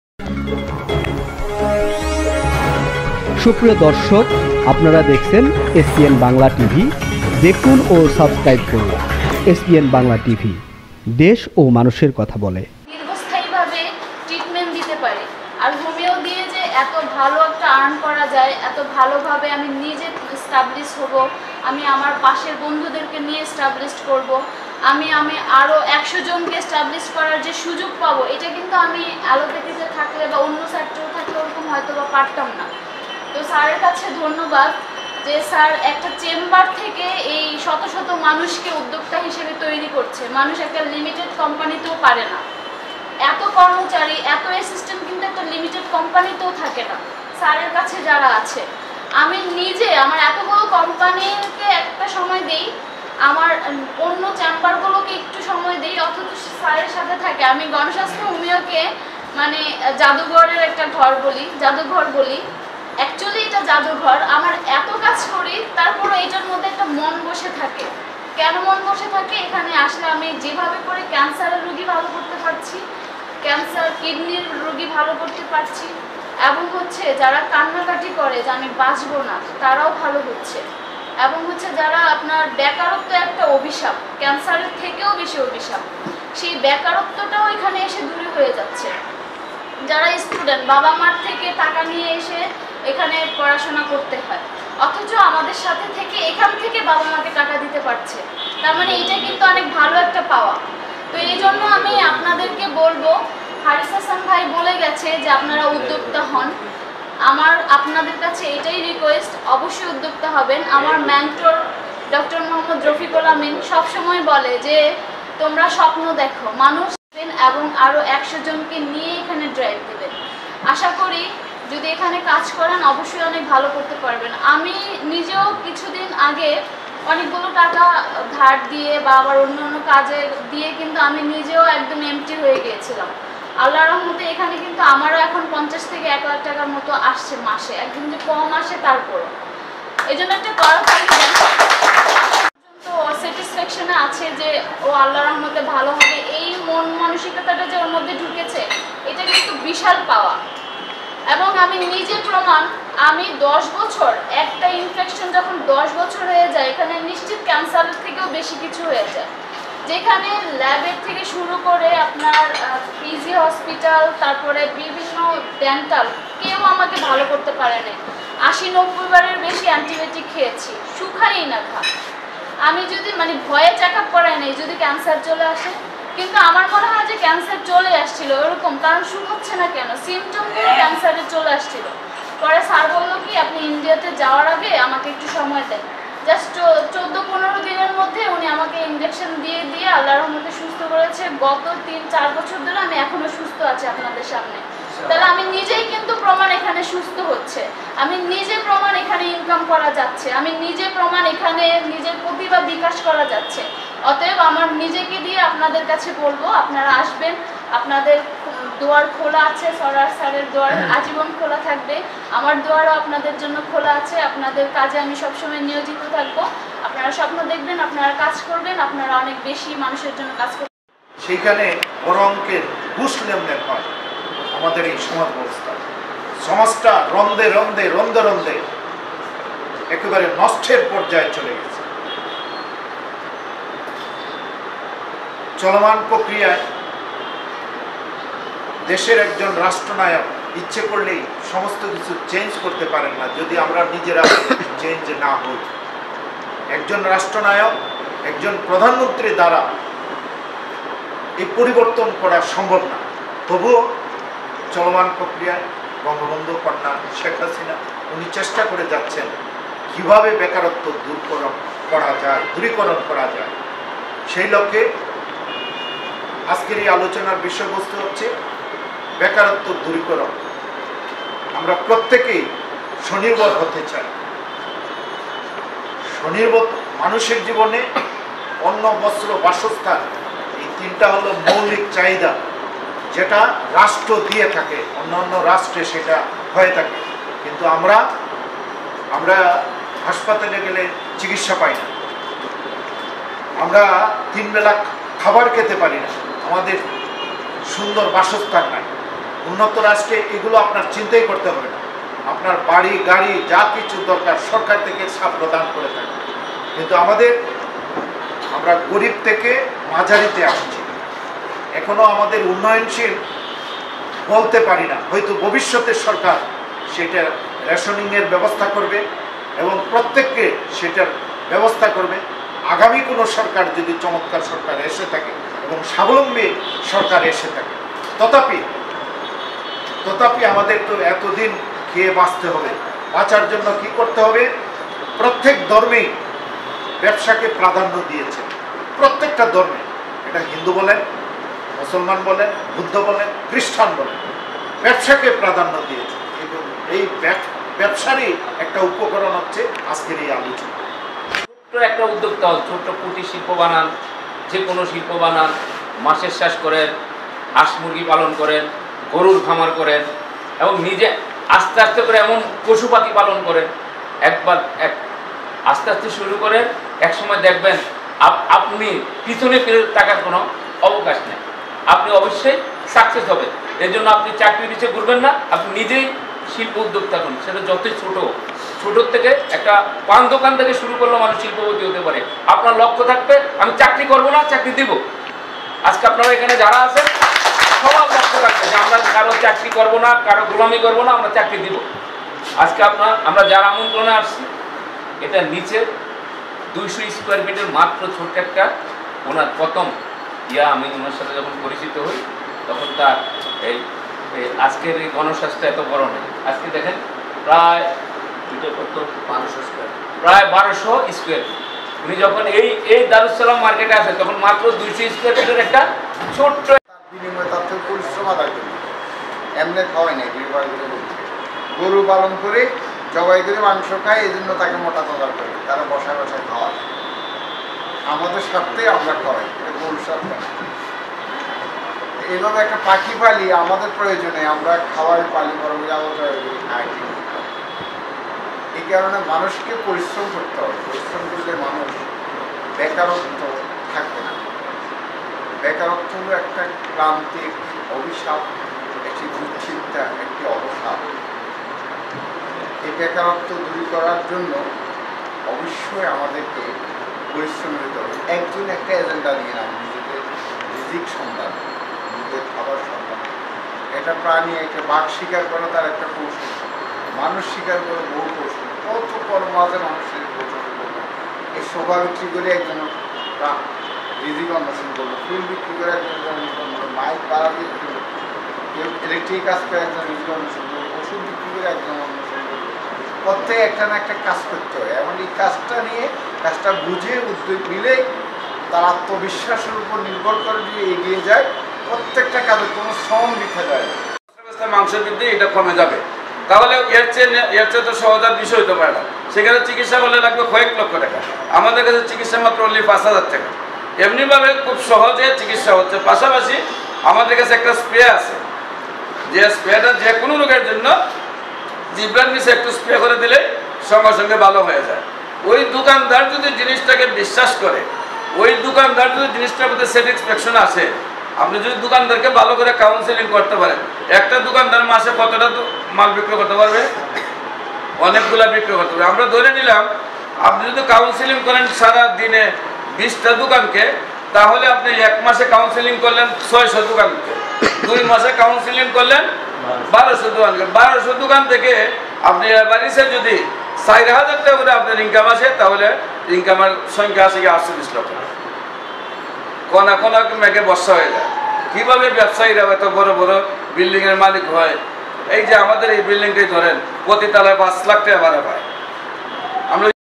বেকারত্ব দূরীকরণ শীর্ষক আলোচনা সভায় বক্তব্য রাখেন ক্যান্সার গবেষক ক্যান্সার গবেষক ডা: শামীমা সোমা
আপনারা দেখছেন এসপিএন বাংলা টিভি সংবাদ: ২৭ডিসেম্বর ২০২১, ১৪ দার উস সালাম আর্কেড গণস্বাস্থ্য হোমিও মিলনায়তনে ৭ম তলা (লিফটের-৬)  গণস্বাস্থ্য হোমিও উদ্যোগে বেকারত্ব দূরীকরণ শীর্ষক আলোচনা সভায় সভাপতিত্ব করেন ও বক্তব্য রাখেন ক্যান্সার গবেষক অধ্যক্ষ ডাঃ এস এম সরওয়ার, চেয়ারম্যান গণস্বাস্থ্য হোমিও প্রধান অতিথি কবি মাহমুদুল হাসান নিজামী সম্পাদক ও প্রকাশক দৈনিক দেজগত, বিশেষ অতিথি হারিস হাসান সাগর, পরিবেশ বিষয়ক সম্পাদক বাংলাদেশ আওয়ামী যুবলীগ, মিঠুন চৌধুরী সভাপতি বাংলাদেশ জনতা পাটি, বিশেষ বক্তা ক্যান্সার গবেষক ডা: শামীমা সোমা, সাধারন সম্পাদক, নিরাপদ জীবন চাই কেন্দ্রিয় কমিটি, ডাঃ মোস্তাফিজুর রহমান, অনুষ্ঠান সঞ্চালনায় আনিসুর রহমান আসিন, প্রমুখ। পরিচালনায়: আজিজুল হক মিন্টু যোগাযোগ:০১৭১১-১৪৩৩৪২।